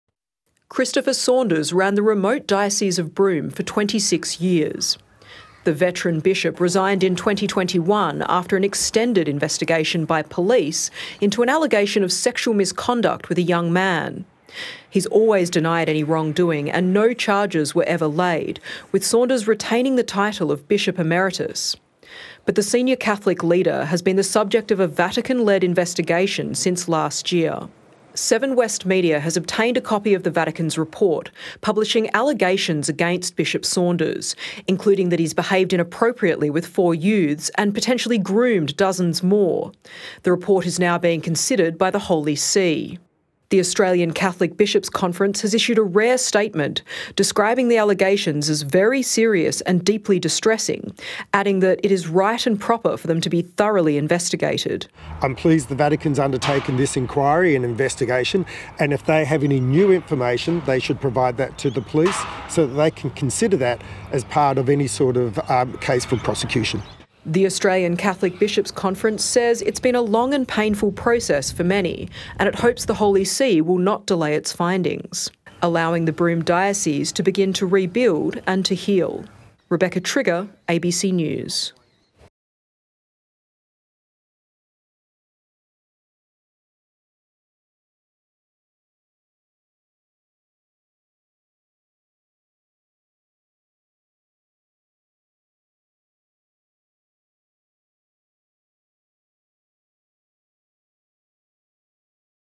Broome Catholic Bishop Christopher Saunders faces sex abuse allegations in Vatican report | ABC News
"Deeply distressing" allegations have been levelled against long-serving Broome Bishop Christopher Saunders, including sexually assaulting young males.